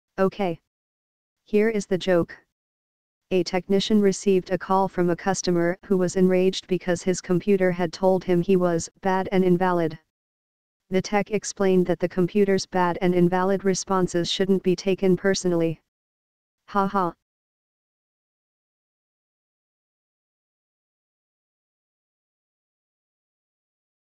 Ok, here is the joke. A technician received a call from a cust...
A technician received a call from a customer who was enraged because his computer had told him he was "bad and invalid". The tech explained that the computer's "bad" and "invalid" responses shouldn't be taken personally.